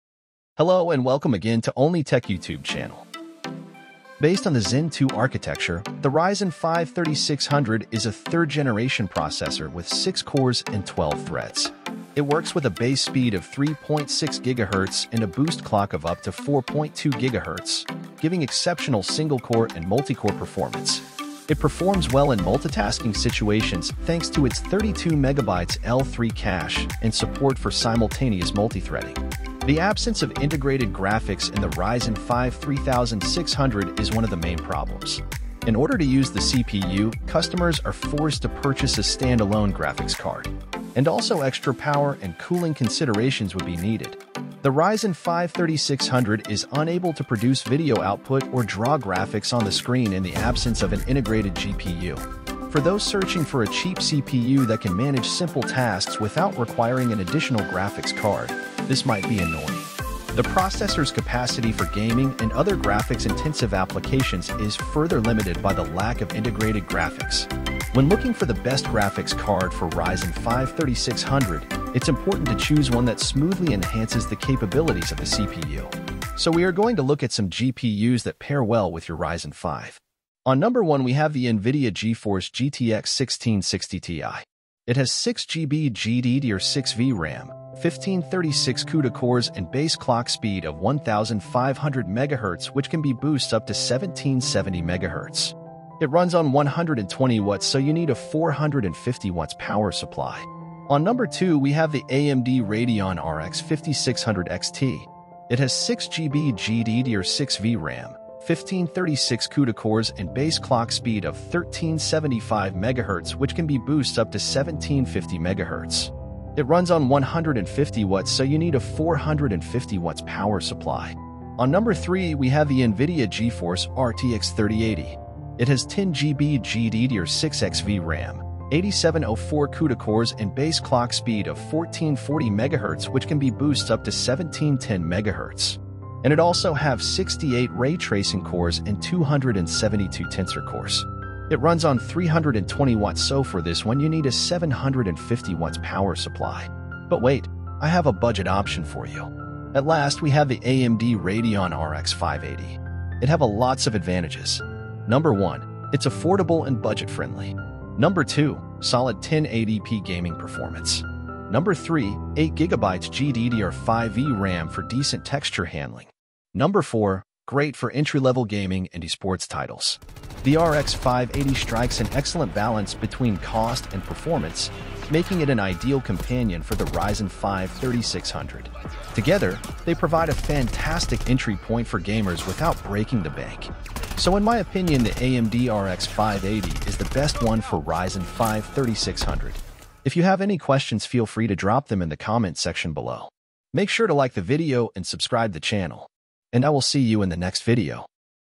Best GPU for Ryzen 5 3600 in 2026 🔥 | RTX 4070 Super vs RX 7800 XT – No Bottlenecks at 1440p!
Timestamps:
00:00 Intro
00:04 Overview to Ryzen 5 3600
01:20 Best GPUs for Ryzen 5 3600
02:30 AMD Radeon RX 580
03:09 Outro

Your Ryzen 5 3600 is still a 2025 champ for budget 1440p gaming—but the right GPU unlocks 100+ FPS without CPU choke. We tested PCIe 4.0 cards: RTX 4070 Super shines with DLSS 3, while RX 7800 XT crushes rasterization for AMD fans. Perfect if you're on AM4 and skipping full AM5 swaps!

2026 Top Picks:
1. RTX 4070 Super → DLSS magic, 120+ FPS at 1440p ($550)
2. RX 7800 XT → Best value, FSR 3 boosts ($500)
3. RTX 3060 Ti → Budget king for 1080p/1440p ($300 used)
4. RX 6700 XT → AMD synergy, great for streaming

Pro Tip: With 16GB RAM, avoid RTX 4090—your 3600 will bottleneck. Upgrade to 5800X3D first if needed!